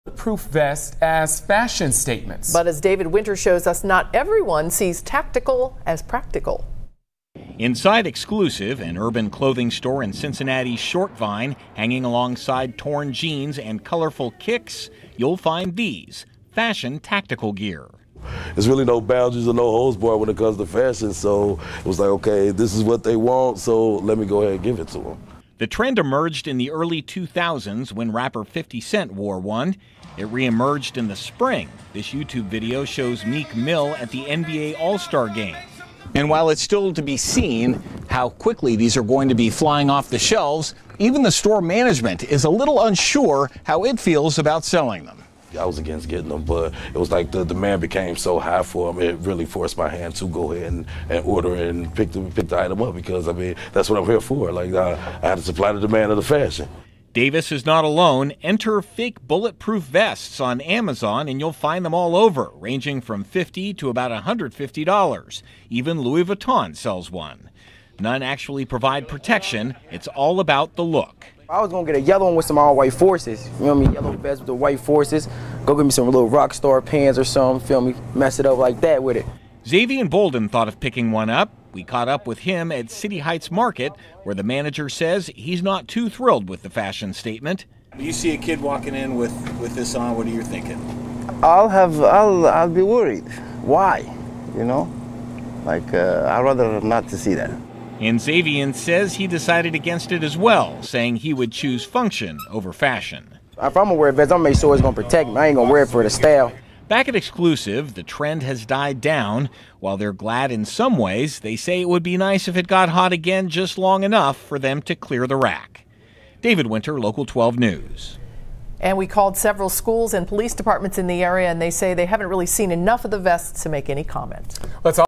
Scary-cool? Fake bulletproof vests worn as fashion statement
CINCINNATI (WKRC) - Young people are strapping on fake bulletproof vests as fashion statements. But not everyone sees "tactical" as practical.

Inside “Exclusive,” an urban clothing store on Short Vine, hanging alongside torn jeans and colorful kicks, you'll find fashion tactical gear.

“There are really no boundaries and no holds barred with fashion,” said Exclusive’s Jay Davis. “So, this is what they want. So let me go ahead and give it to them.”

The trend emerged in the early '00s when rapper 50 Cent wore one. It re-emerged in the spring with rapper Meek Mill wearing one while performing at the NBA All-Star game. And while it’s still to be seen how quickly the faux tactical vests will continue to fly off the shelves, even store management is a little unsure of how they feel about selling them.

“I was against getting them,” said Davis. “But the demand was so high, it really did force my hand and order them and pick the item up. That’s what I’m here for –- I have to supply the demand of the fashion.”

Davis isn't alone; just enter "fake bulletproof vests" on Amazon. You'll find them all over, ranging from $50 to about $150. Even Louis Vuitton sells one. None of them actually provide protection. It's all about the look.

Twenty-year-old Xavian Bolden thought of picking one up.

“I was gonna get a yellow one with all white Forces (shoes). Get me some rock star pants. Feel me?" said Bolden.

Local 12 caught up with him at City Heights Market where the manager says he's not too thrilled with the fashion statement.

“I’ll be worried [if someone walked in with one],” said manager Sam Tibtani. “Why? You know, I’d rather not to see that.”

Bolden says he decided against investing in tactical wear. He would choose function over fashion.

“If I’m gonna wear a vest, I’m gonna wear one that’s gonna protect me. I ain’t gonna wear it for the style,” he said.

Back at Exclusive, the trend has died down. While they say they're glad in some ways, they also say it would be nice if the trend got hot again just long enough for them to clear the rack.

Local 12 called several schools and police departments in the area, and they say they haven't seen enough of the vests to comment.